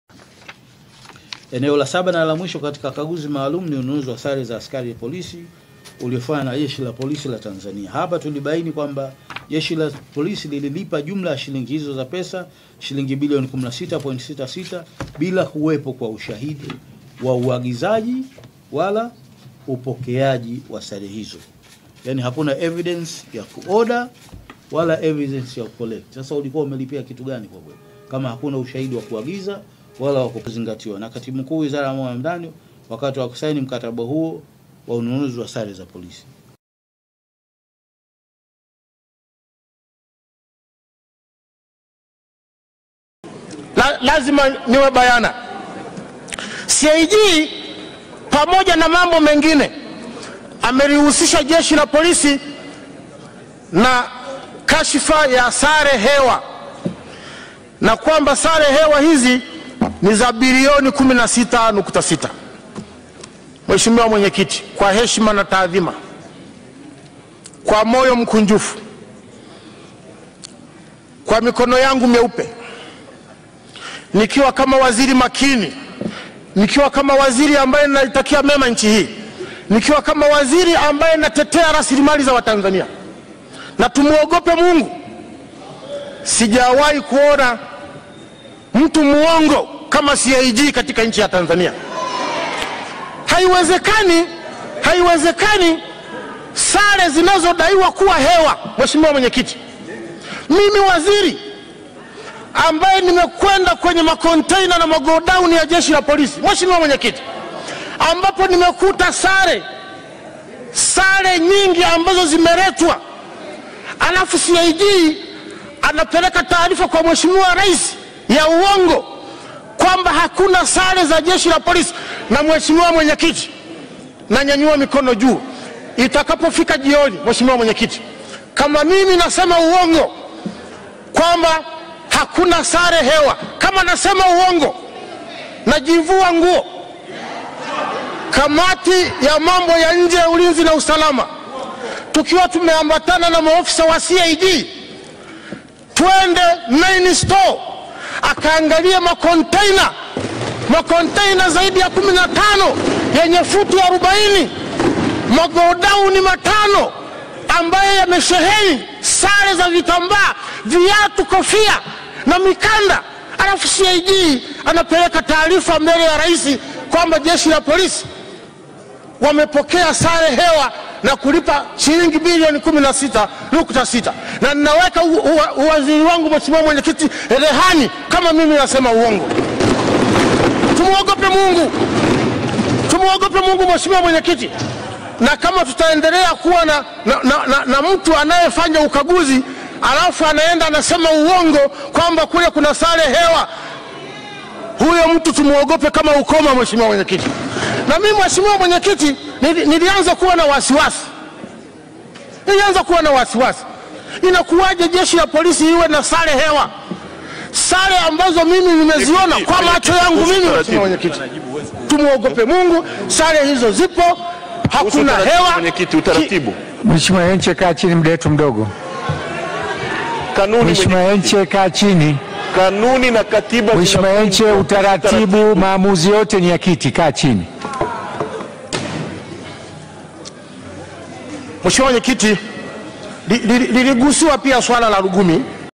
Kangi Lugola Ametumwa na Nani?
Akitoa Hotuba ya Wizara ya Mambo ya ndani ya Nchi, waziri Kange Lugola ameliambia bunge yakuwa atajiuzulu nafasi yake ya uwaziri endapo itabainika kuwa sare za Jeshi la Polisi hazipo "CAG muongo na sijawahi kumuona mtu muongo Tanzania kama CAG" alisema Lugola